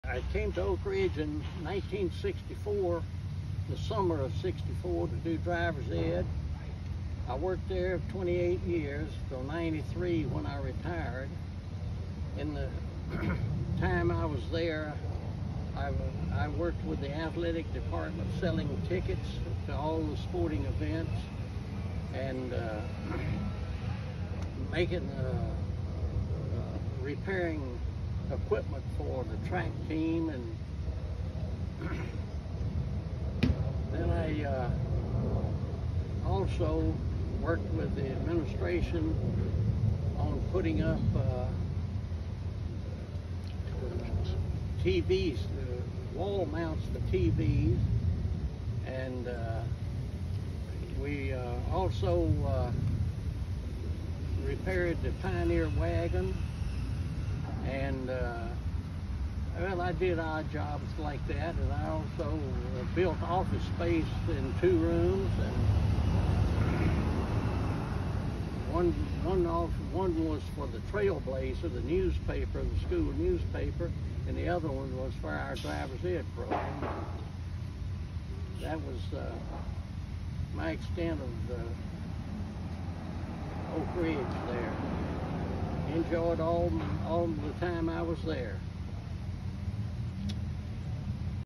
*Jim Koffman - Oak Ridge Athletic Hall of Fame - Class of 2021
Jim Koffman (Contributor) - Spent 28 years of his 32 years of teaching at Oak Ridge High School (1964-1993) serving the athletics program in several capacities. He helped sell football and basketball game tickets for the Pioneers for 25 seasons.  He went on to build the famous Pioneer Wagon in 1992 that stood outside the main office and now is in the courtyard since the rebuild of the school in 2013. In his dedication to the ORHS track & field program, Mr. Koffman built the jumping boxes and platforms used for the training of the long and triple jumpers, as well as filming and editing training videos for the hurdlers, jumpers and relay exchanges.  The quality of the videos was such that the coaches used them when speaking at coaching clinics.  His other contributions to Oak Ridge included him building the offices for Miss Lockaby and the Yearbook Staff, as well as the offices for the Driver Education program.